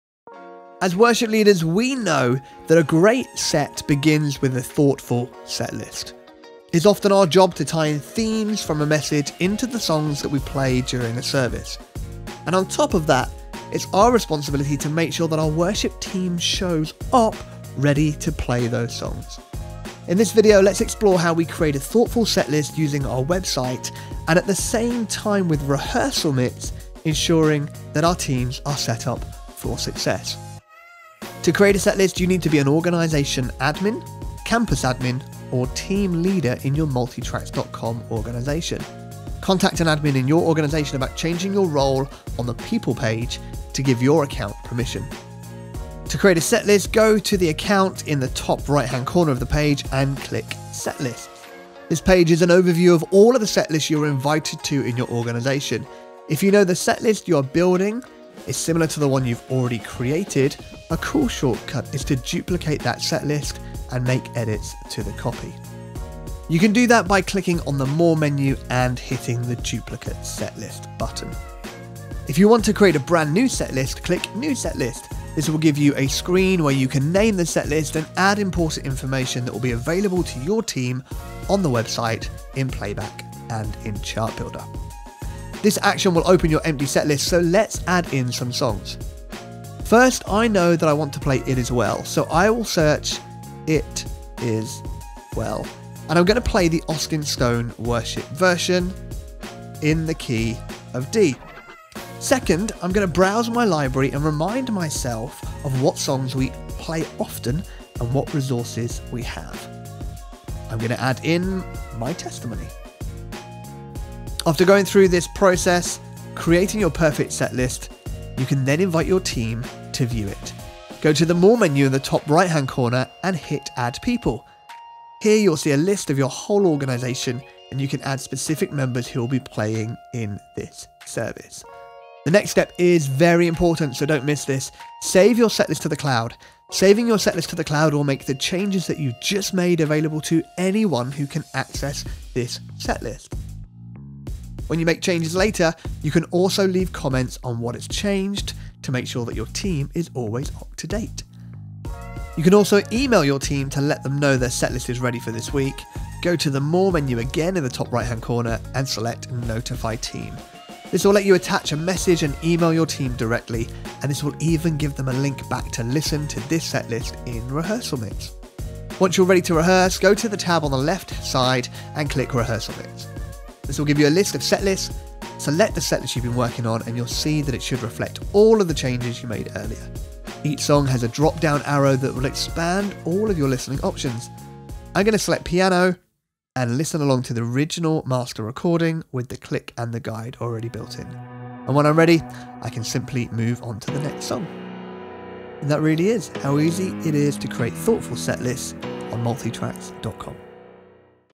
HOW TO | Create Setlists Online
Create a setlist, add songs, and notify your team all from your account tab on MultiTracks.com. Save changes to the cloud and everyone will stay up to date whether they view the setlist online, in Playback, or in ChartBuilder. With a RehearsalMix subscription, members of your team can practice their parts for the upcoming setlist.